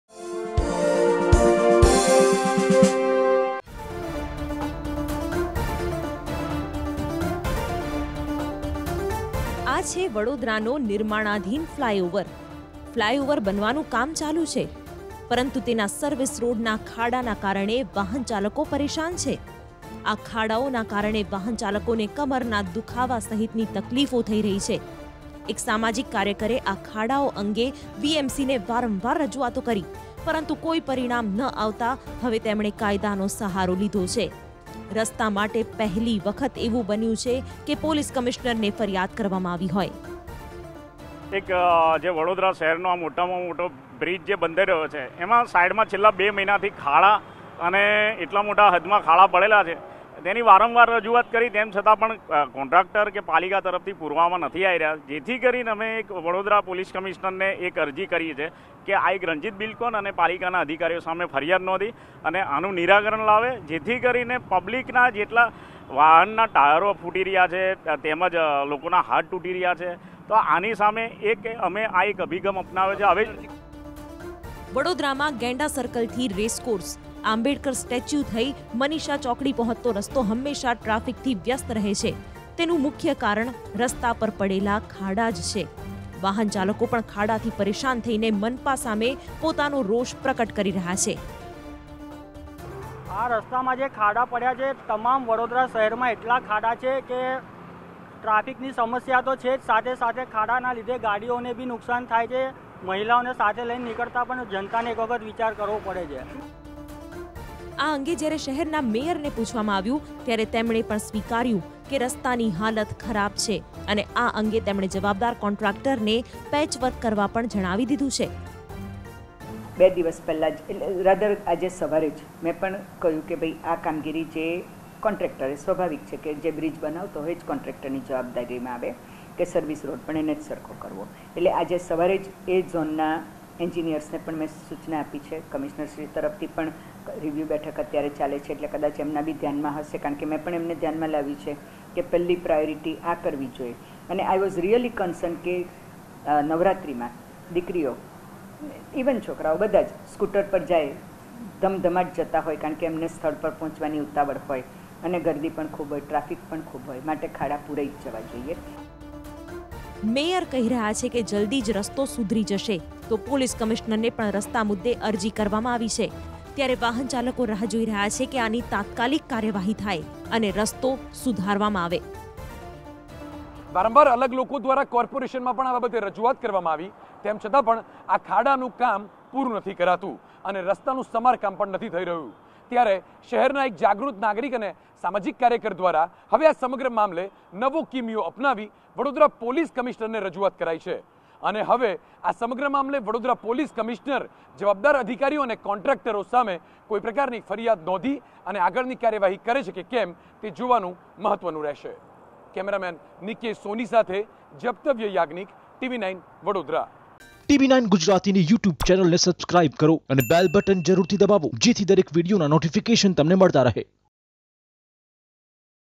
Potholed service lanes irk commuters , Vadodara | Tv9GujaratiNews
Potholed service lanes irk commuters , Vadodara